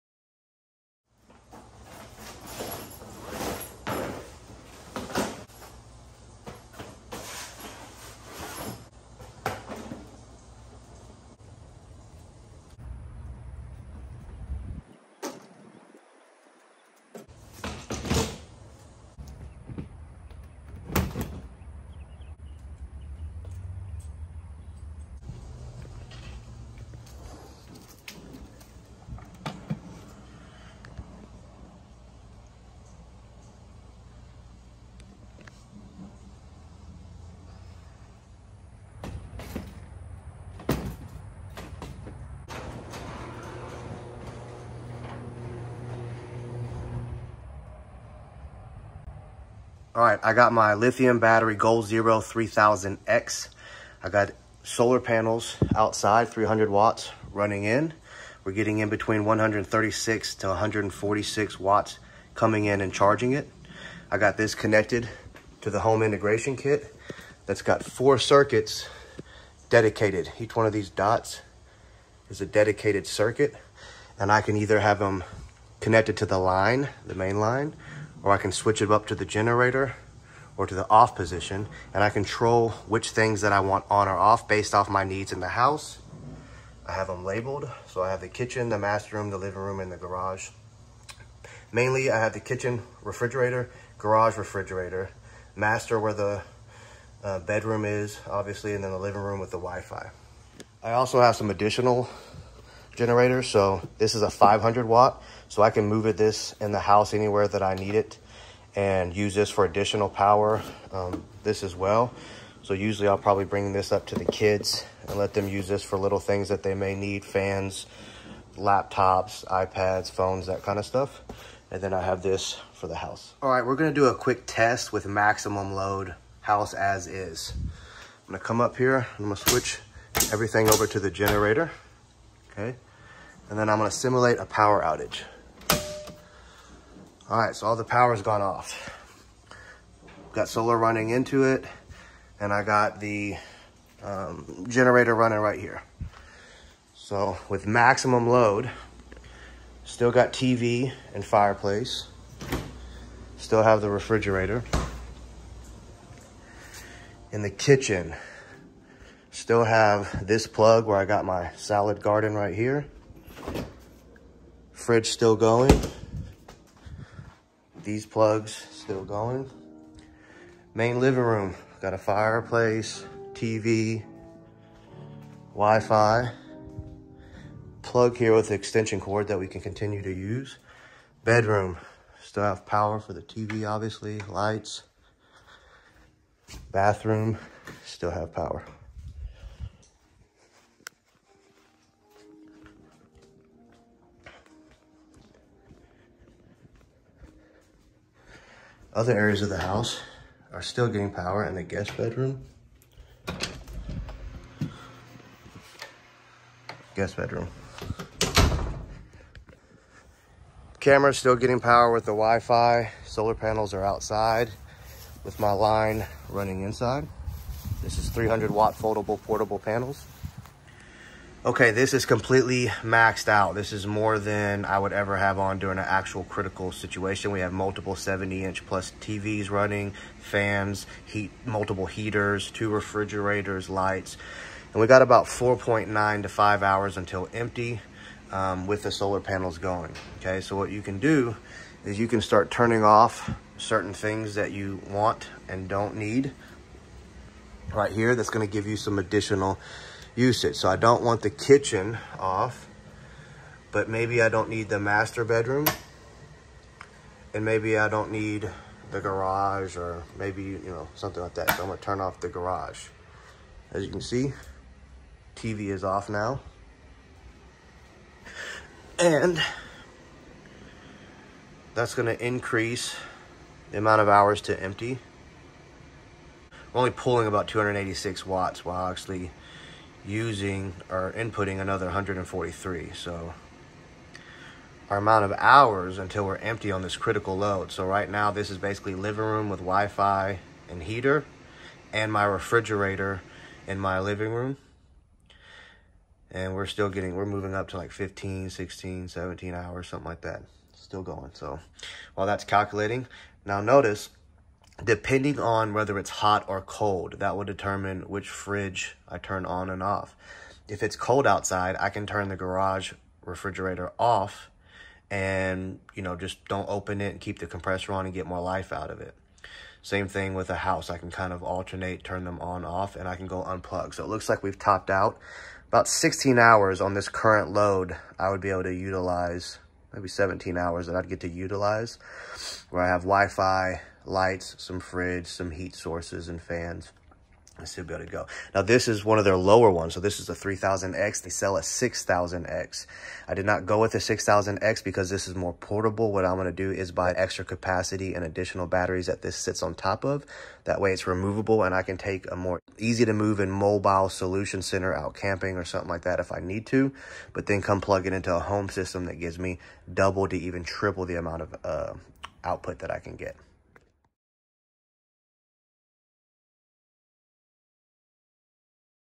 Goal Zero 3000x / Ranger 300 Emergency Test
Goal Zero 3000x lithium battery generator paired with Ranger 300 solar panels emergency test.